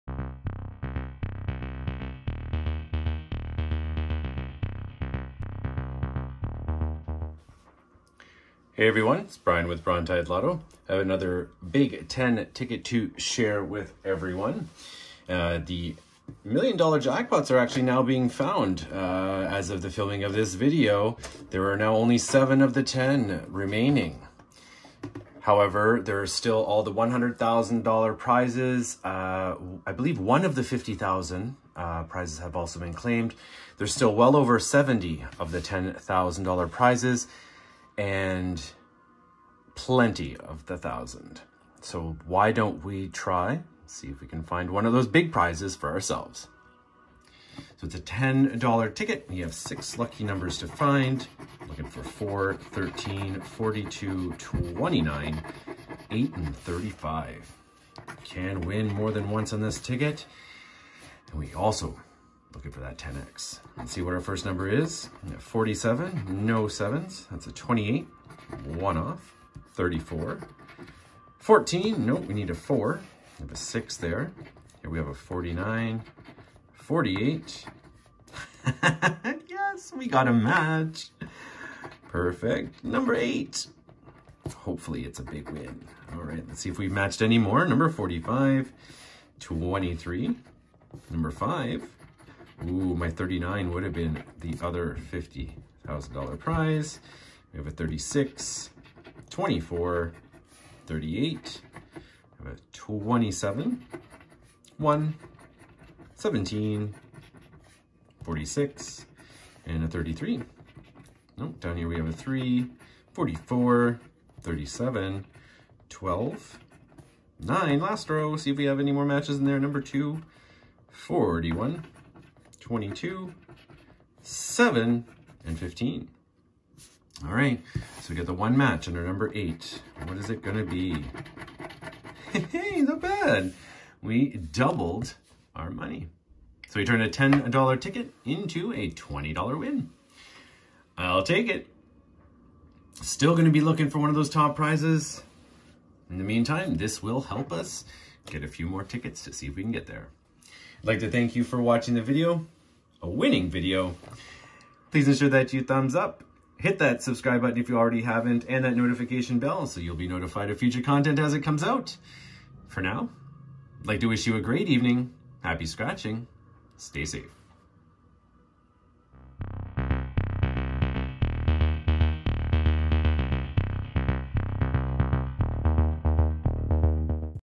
OLG BIG10 TICKET! 7 TOP PRIZES REMAIN!
The shiny tickets are fun and the top prizes are slowly being found. Looking for my piece of that Jackpot pie!

Play Responsibly and within your limit.